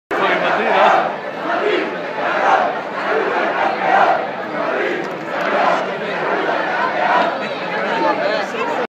FC Barcelona - Atl Madrid super cup 28. aug 2013 - Madrid Cabron!!
Barcelona fans after game